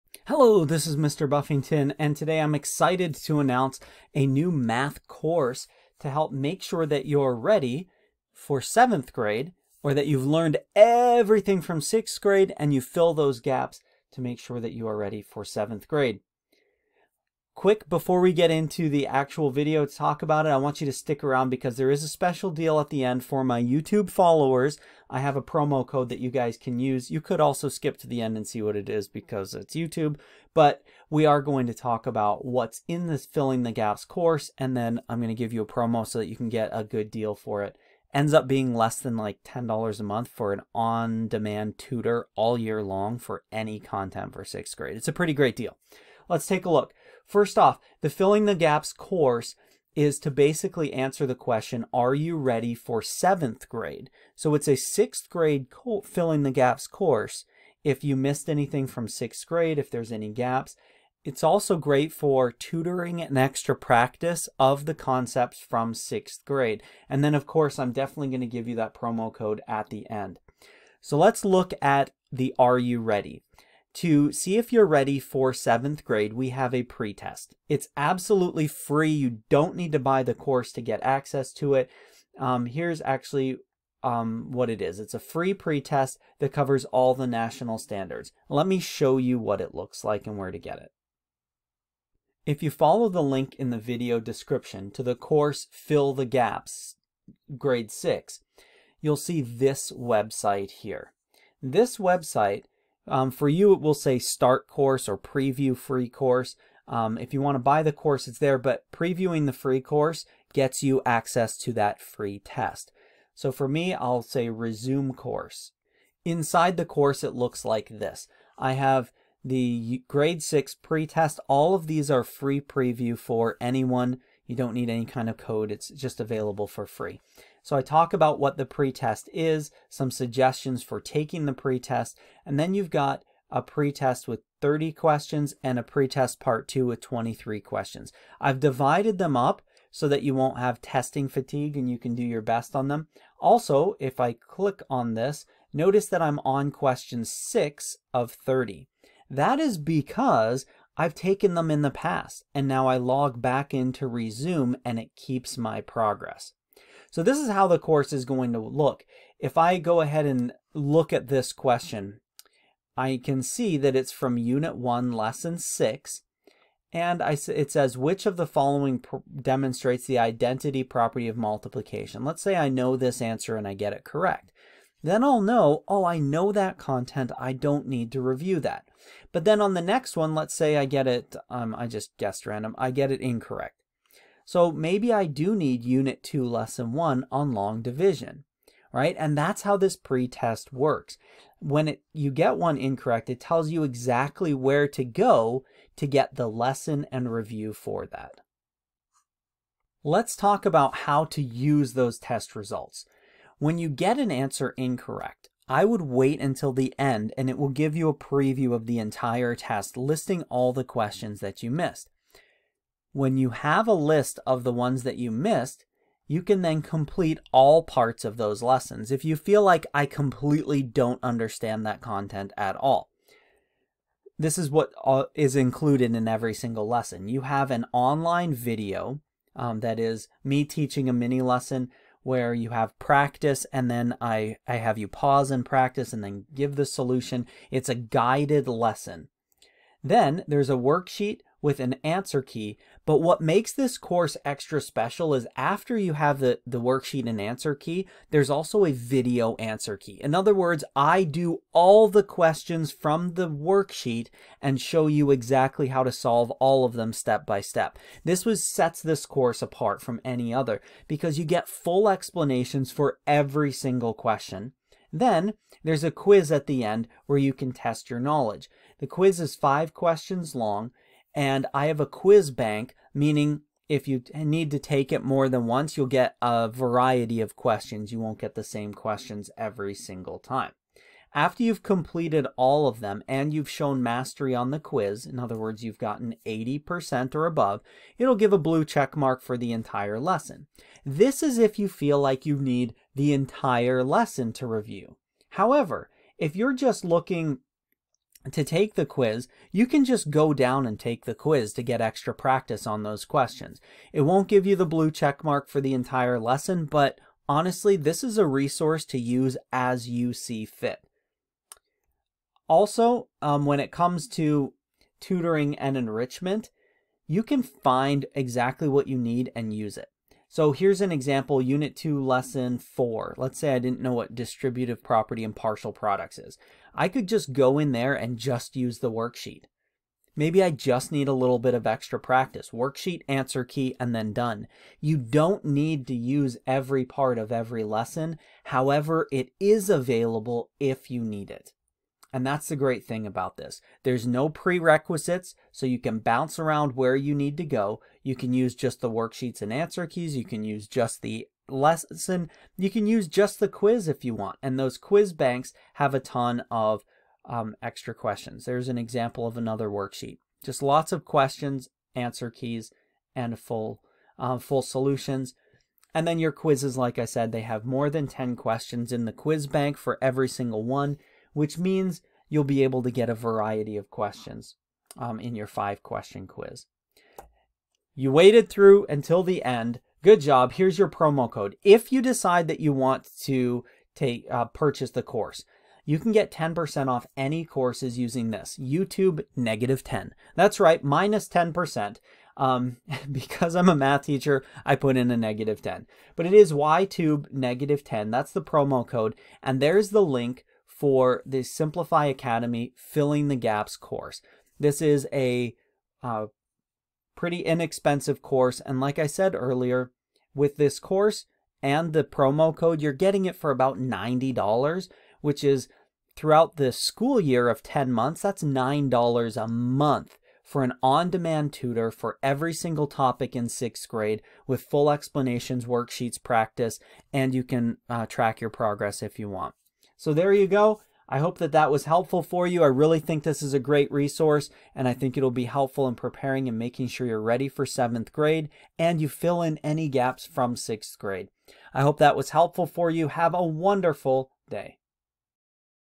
Ready for 7th Grade Math? | Free Test & Discount on a Full Course!
Are you ready for 7th grade mathematics? Take a free assessment to find out if there are any gaps in your 6th grade math knowledge. This quick test covers all the standards from 6th grade and is a good indicator of your readiness for 7th grade.

  You will have access to the entire course for a full year, in other words, you will have a certified teacher on-demand for any topic from 6th grade any time you need for less than $10 per month!